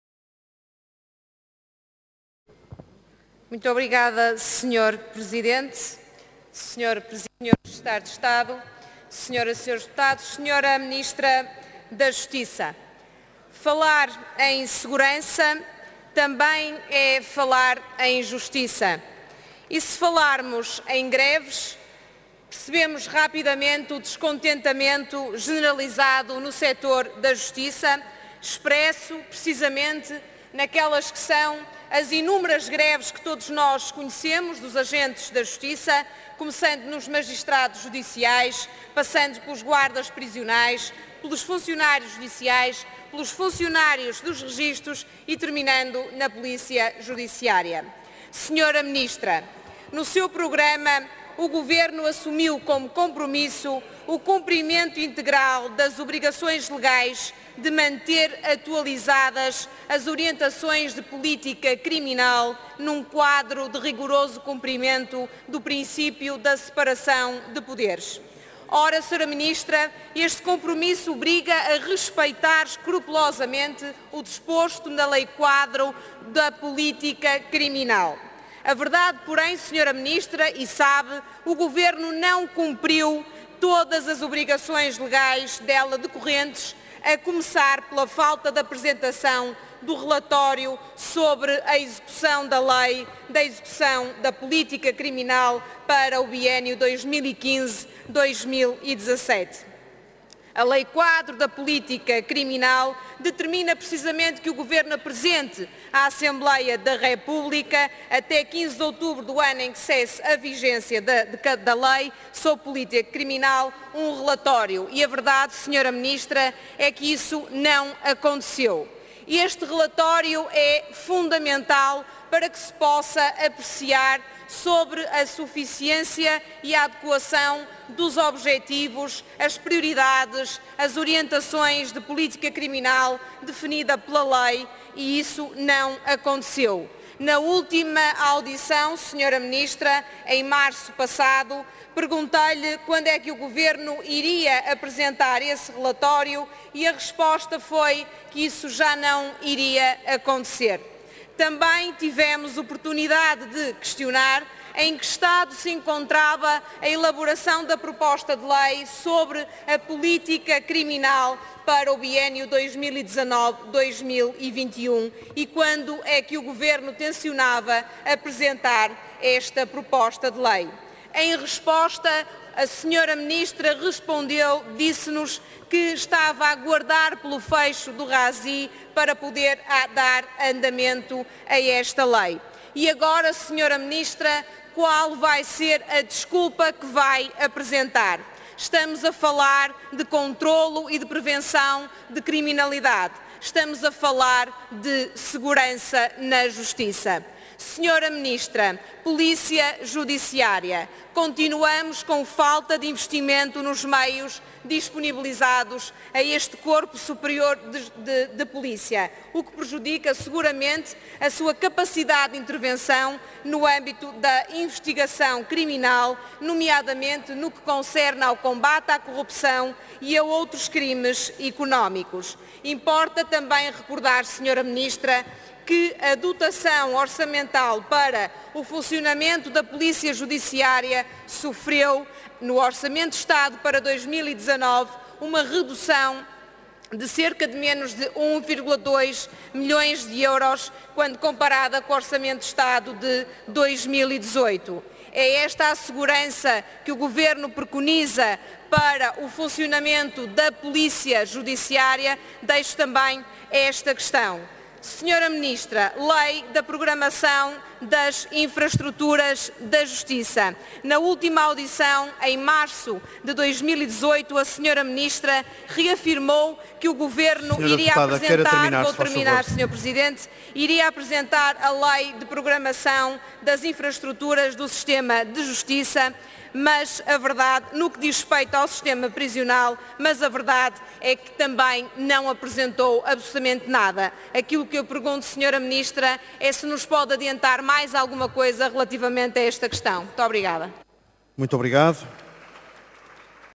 Andreia Neto no debate sobre condições do exercício da segurança em Portugal
Intervenção do deputado no debate, agendado pelo PSD, sobre "as condições do exercício da segurança em Portugal".